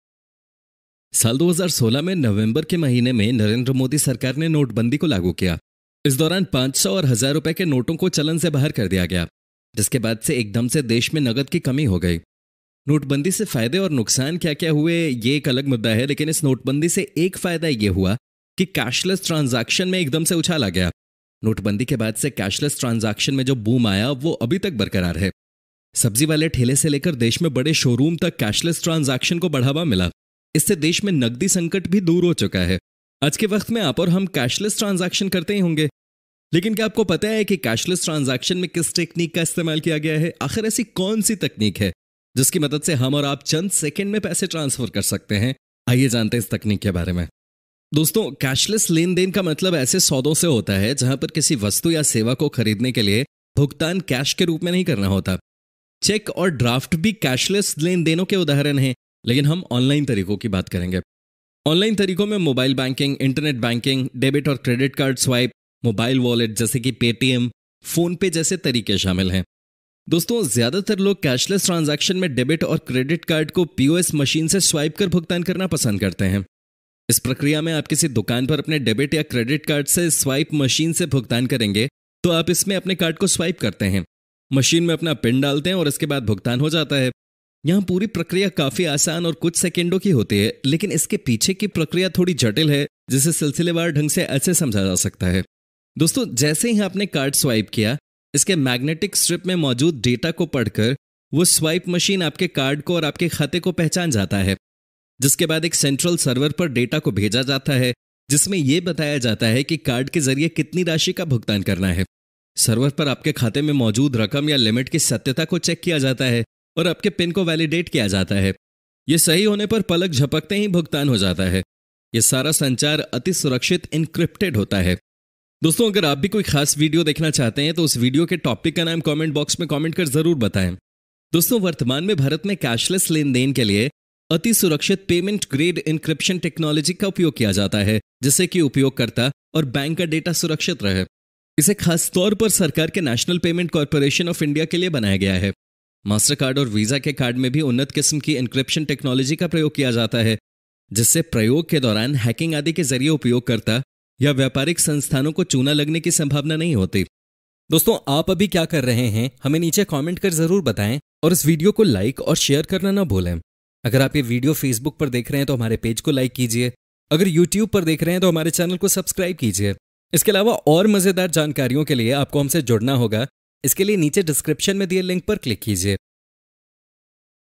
कैसे होता स्वाइप मशीन से कैशलेस ट्रांजेक्शन? किस तकनीक का होता है इस्तेमाल?
साल 2016 में नवंबर के महीने में नरेंद्र मोदी सरकार ने नोटबंदी को लागू किया. इस दौरान 500 और हजार रुपए के नोटों को चलन से बाहर कर दिया गया. जिसके बाद से एकदम से देश में नकद की कमी हो गई. नोटबंदी से फायदे और नुकसान क्या-क्या हुए ये एक अलग मुद्दा है लेकिन इस नोटबंदी से एक फायदा ये हुआ कि कैशलेस ट्रांजेक्शन में एकदम से उछाल आया. नोटबंदी के बाद से कैशलेस ट्रांजेक्शन में जो बूम आया वो अभी तक बरकरार है. सब्जी वाले ठेले से लेकर देश में बड़े शोरूम तक कैशलेस ट्रांजेक्शन को बढ़ावा मिला. इससे देश में नकदी संकट भी दूर हो चुका है. आज के वक्त में आप और हम कैशलेस ट्रांजेक्शन करते भी होंगे. लेकिन क्या आपको पता है कि कैशलेस ट्रांजेक्शन में किस टेक्निक का इस्तेमाल किया गया है? आखिर ऐसी कौनसी तकनीक है जिसकी मदद से हम और आप चंद सैकेंड में पैसे ट्रांसफर कर सकते हैं? आइए जानते हैं इस तकनीक के बारे में.

दोस्तों, कैशलेस लेनदेनों का मतलब ऐसे सौदों से होता है जहां पर किसी वस्तु या सेवा को खरीदने के लिए भुगतान कैश के रूप में नहीं होता है. चेक और ड्राफ्ट भी कैशलेस लेनदेनों के उदाहरण है लेकिन हम ऑनलाइन तरीकों की बात करेंगे. ऑनलाइन तरीकों में मोबाइल बैंकिंग, इन्टरनेट बैंकिंग, डेबिट और क्रेडिट कार्ड स्वाइप, मोबाइल वॉलेट जैसे कि Paytm, PhonePe जैसे तरीके शामिल हैं.